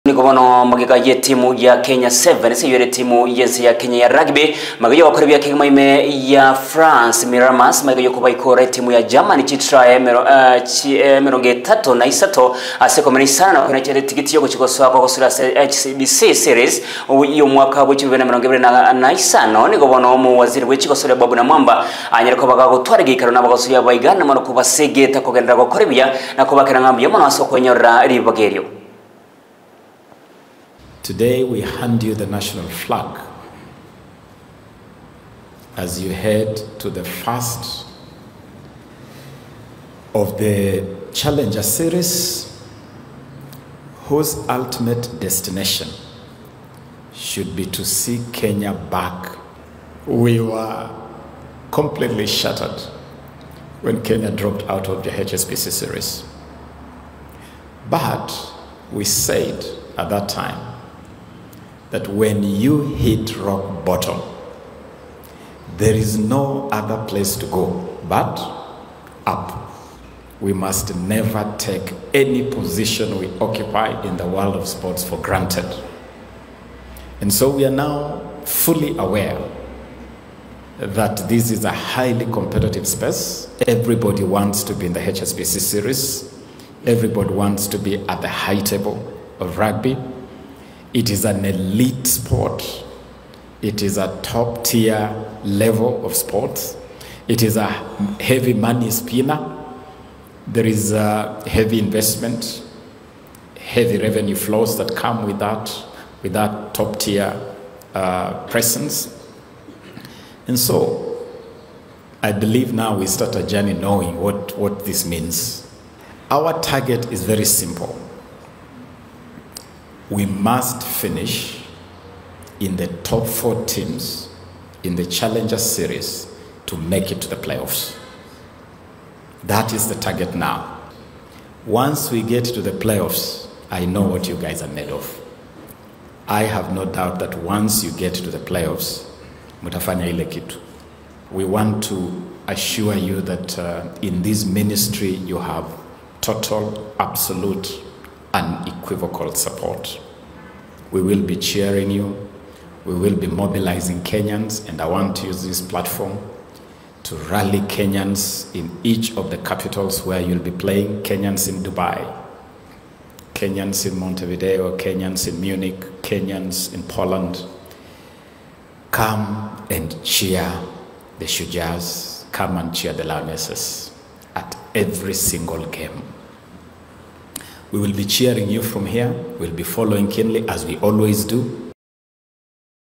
Waziri Ababu Namwamba amewapa hongera timu ya Kenya Sevens kwa kufanya vizuri HSBC sevens.
Waziri wa michezo Ababu Namwamba amewapa hongera timu ya Kenya Sevens kwa kufanya vizuri kuingia kwenye HSBC Sevens Series 2025